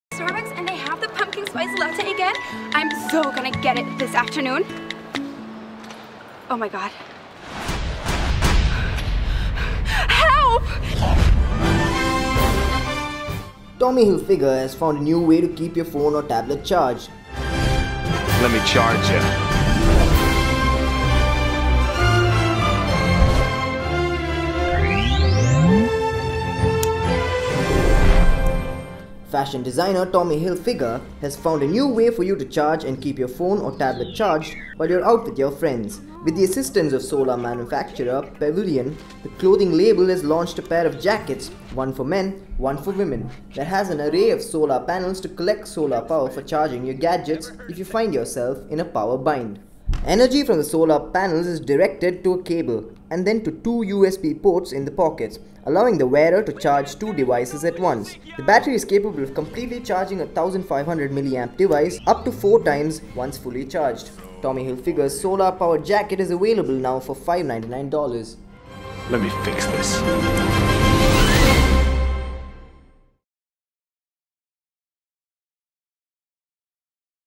Tommy Hilfiger launches Solar Power Jackets to charge your phone
Fashion designer Tommy Hilfiger has found a new way for you to keep your phone or tablet charged while you're out with your friends. With the assistance of solar manufacturer Pvilion, the clothing label has launched a pair of jackets one for men, one for women that has an array of solar panels to collect solar power for charging your gadgets if you find yourself in a power bind.

Energy from the solar panels is directed to a cable and then to two USB ports in the pockets allowing the wearer to charge two devices at once.

The battery is capable of completely charging a 1,500 mAh device up to four times once fully charged. Tommy Hilfiger's solar-powered jacket is available now for $599.